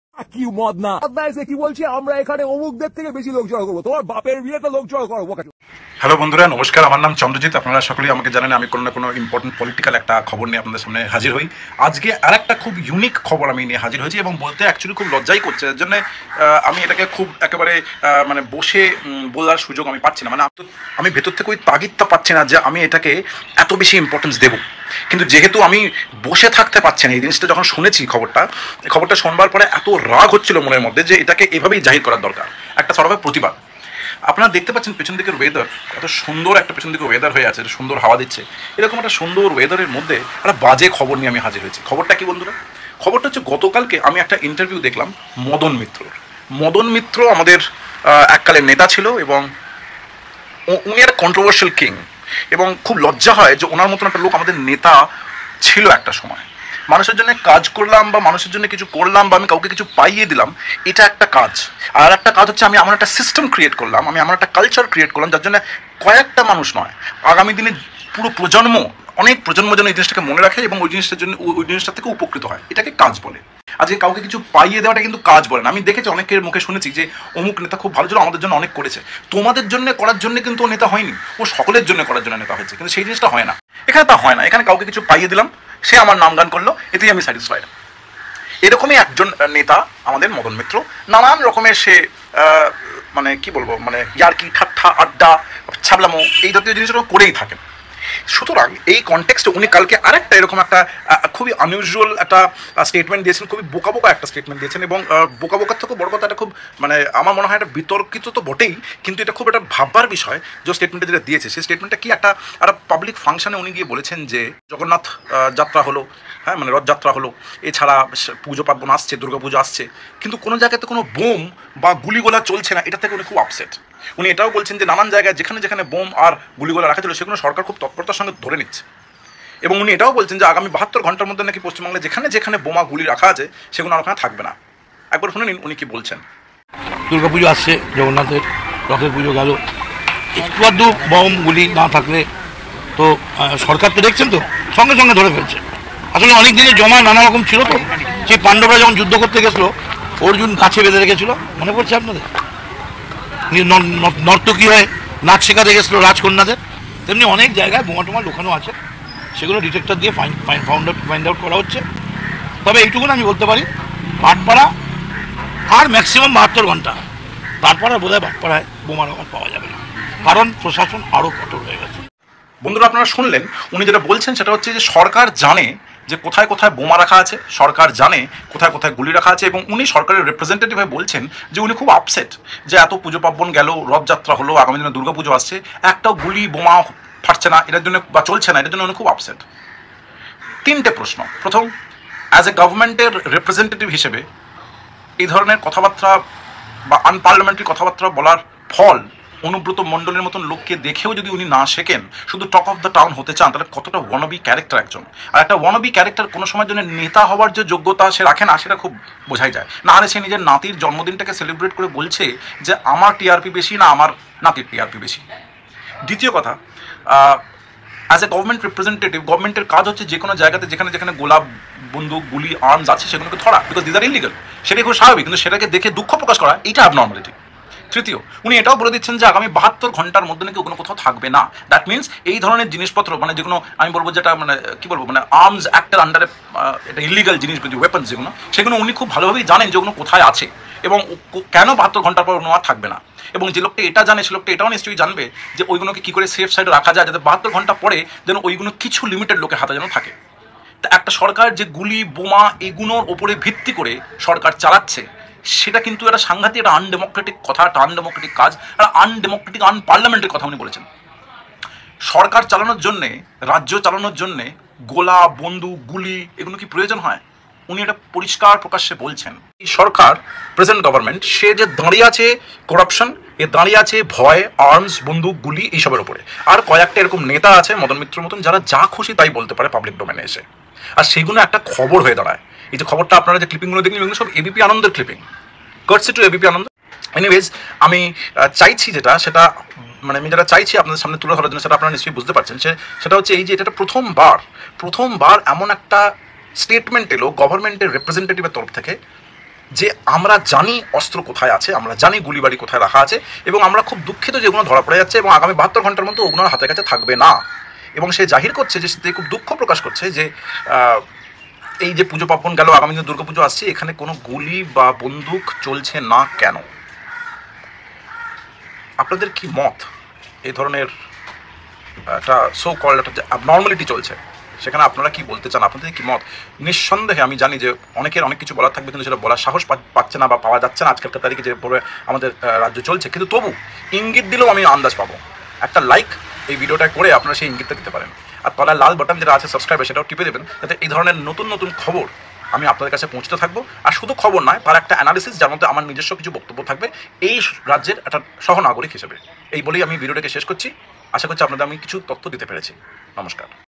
Madan Mitra: "একটু বোমা-গুলি না থাকলে তো…'' | ChandrajitVLOGS
'একটু বোমা-গুলি না থাকলে তো...'' মদনের মন্তব্যে তোলপাড়
দুর্গাপুজো আসছে, জগন্নাথের রথেরও পুজো গেল। একটু বোমা-গুলি না থাকলে তো...সরকার দেখছে। অনেকদিন ধরে জমে ছিল, সরকার দেখছে, ধরে ফেলছে। অনেক জায়গায় জমা, খুঁজে বের করা হচ্ছে। আর ৭২ ঘণ্টা পরে আর বোমা পাওয়া যাবে না।'' মদনের মন্তব্যে তোলপাড়।